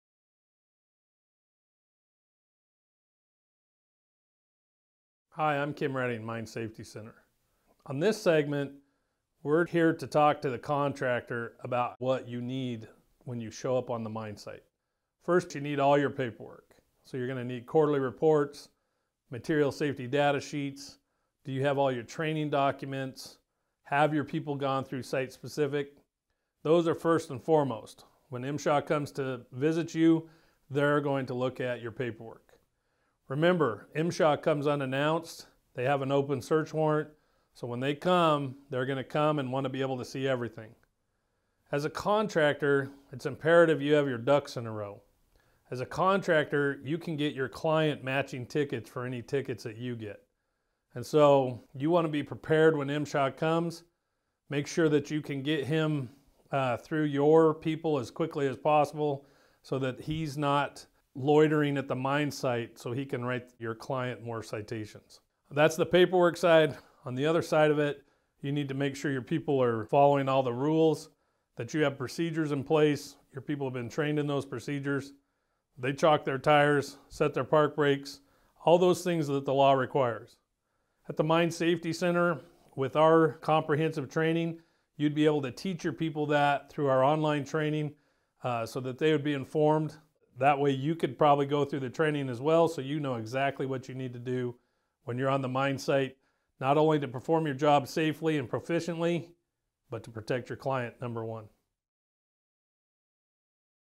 MSHA Compliance - Contractor Requirements
MSHA training and paperwork are only a part of mining contractor requirements.